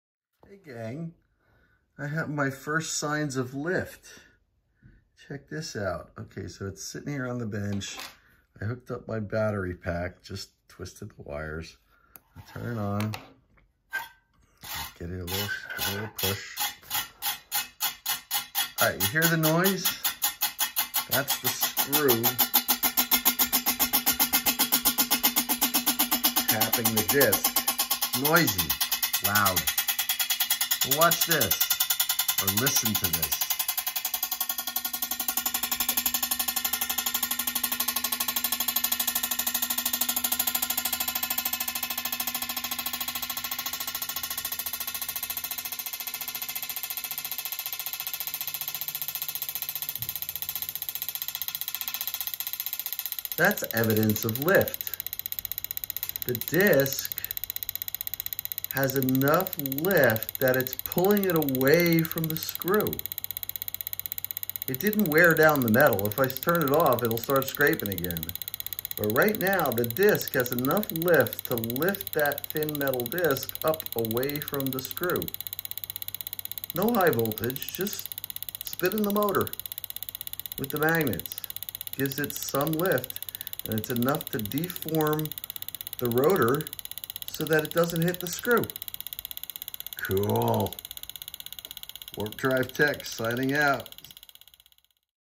First Evidence of Lift!
Exciting isn't it? It's not much but it's like that little flag on the golf course. It gives you hope!

WARNING! Safety First! = Spinning magnets around at high speed is extremely dangerous. If a Neodymium magnet shatters, the pieces are propelled by extremely strong fields pressures, in addition to the centripetal force of the spinning mass. The pieces can cut through tissue like a shotgun. In addition, while a little static electricity is fun to play with, this project is using a 30kV Flyback transformer from a TV set, powered by a battery, that as a system can provide several hundred watts of HV power. Enough to kill a man stone dead instantly! Putting the HV in a protected box, with no access to the HV other than through an insulated wire, is for my own safety and those around me.

Support new content and Experiments!
Thank you for the generous support we have received!